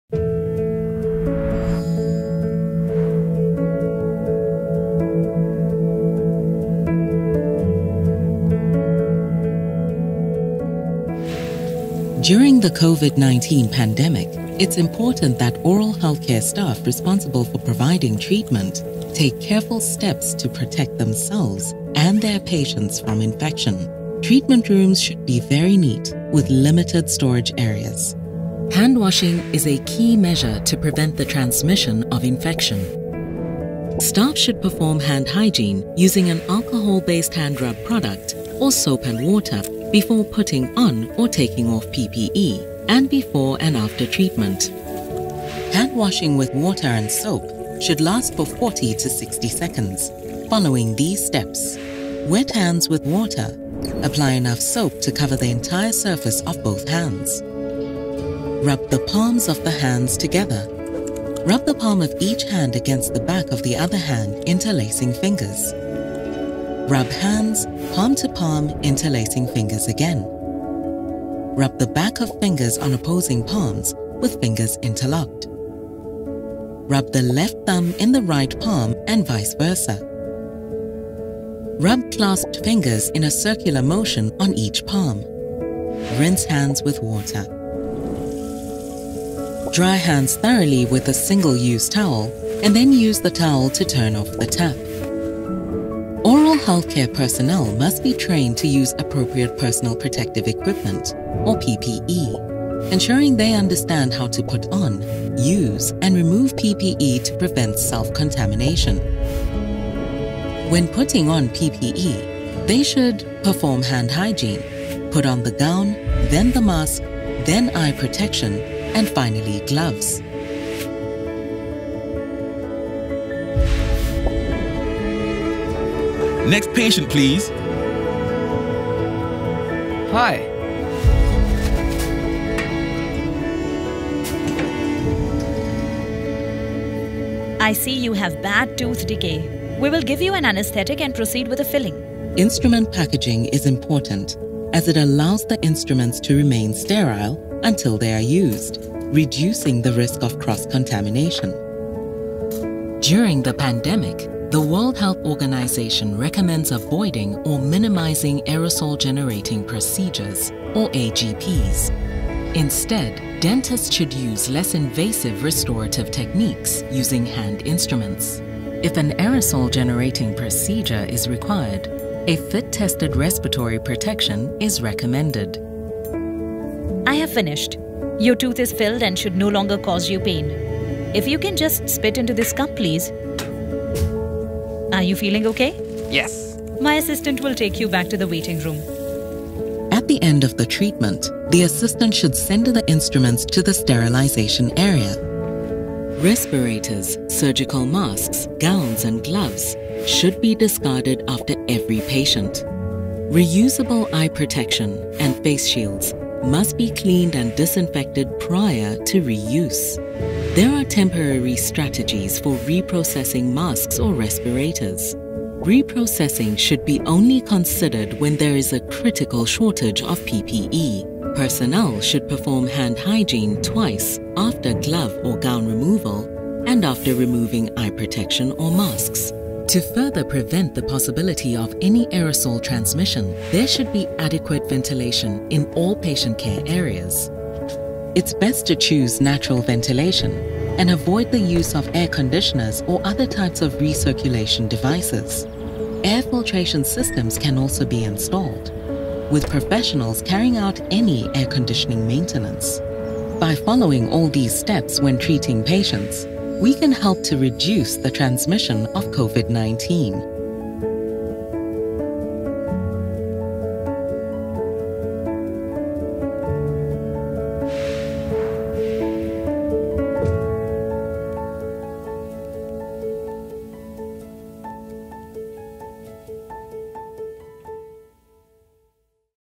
Oral health services during COVID-19: protecting patients and personnel during treatment (video 2)
This short video explains World Health Organization recommendations for protecting both patients and oral health-care personnel from infection with the COVID-19 virus during oral health and dental treatment during the COVID-19 pandemic.

The video is primarily intended for use in trainings for oral health teams on infection prevention and control. More generally, target audiences are public health authorities, in particular those responsible for oral health, and oral health-care personnel undertaking or assisting in the procedures described, working in both the public and private sectors. People considering making an appointment for oral health and dental care may also find it useful as a reference for the practices that they should expect to see in oral and dental health-care centres during the pandemic and beyond.